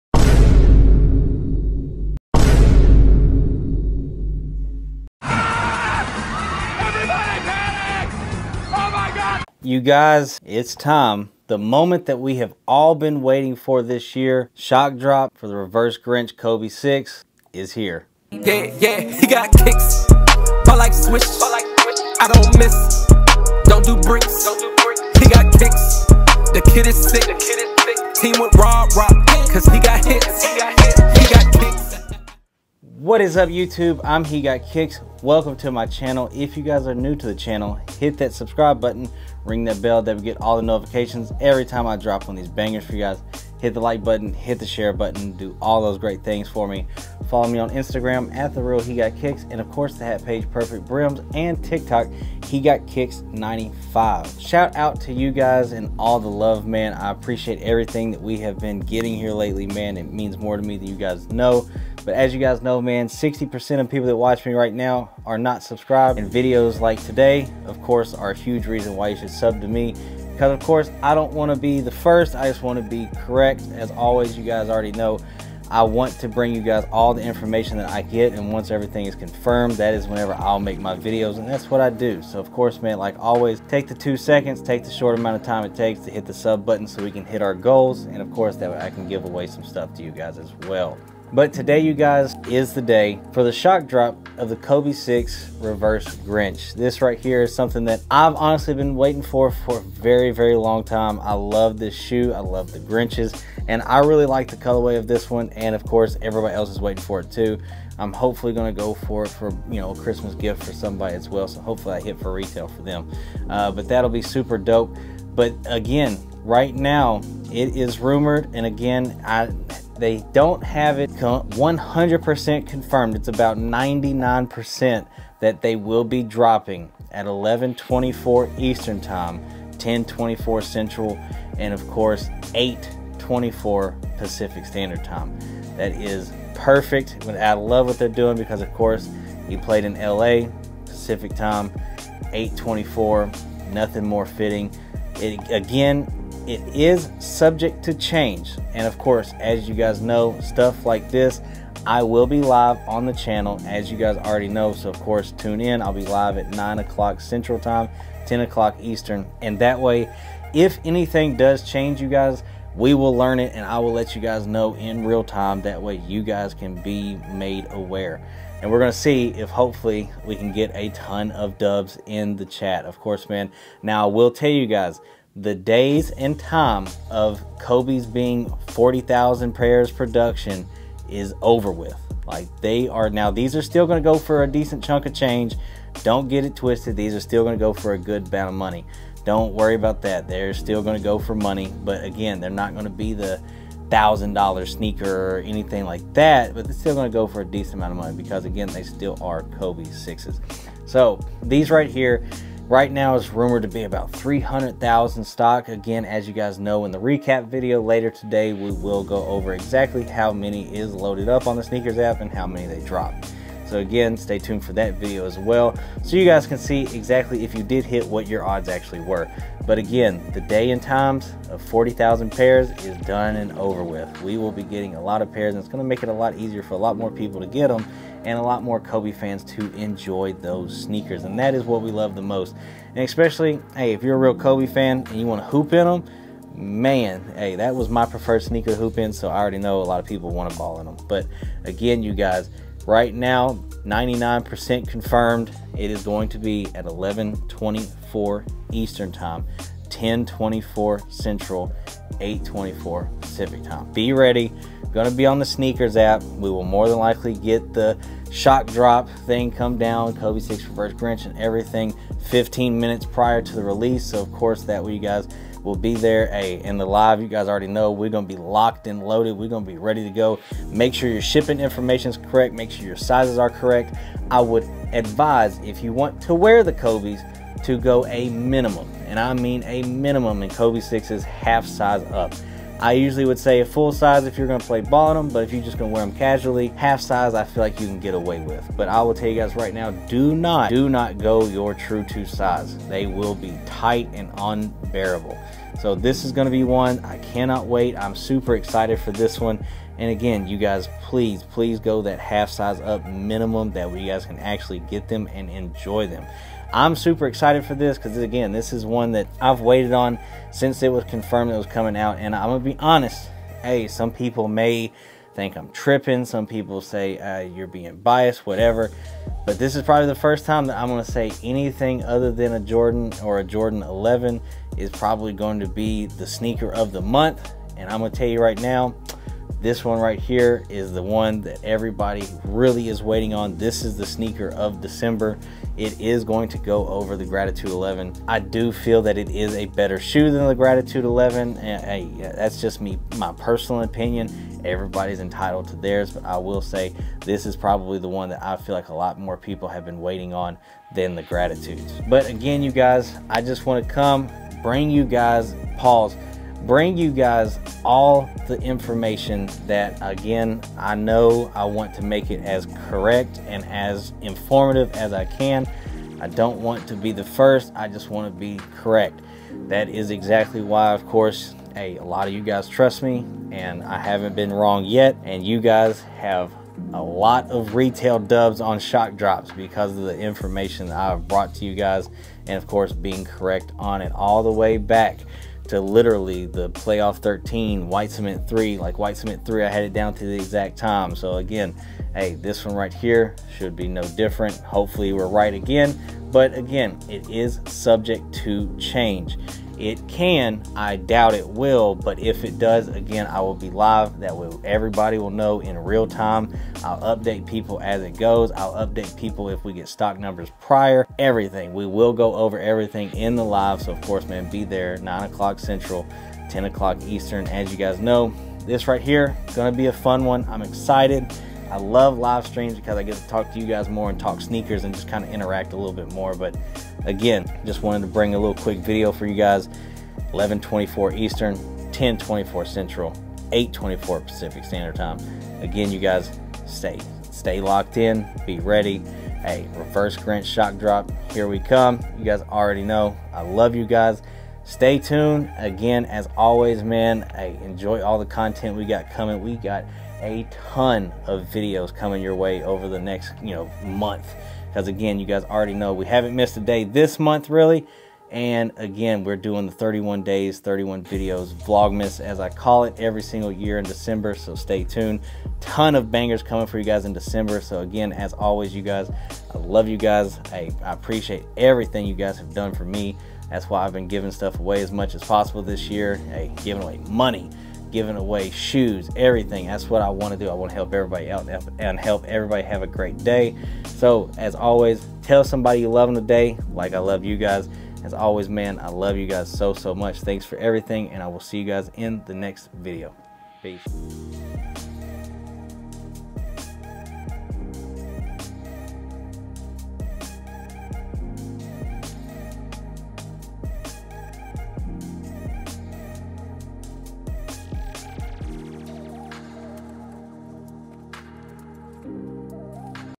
SHOCK DROP CONFIRMED FOR THE KOBE 6 "REVERSE GRINCH" EVERYTHING YOU NEED TO KNOW TO COP FOR RETAIL!
BEST TRAVEL BAG FOR SNEAKERHEADS

OUTRANK

Discount Hegotkickz

Perfectbrimz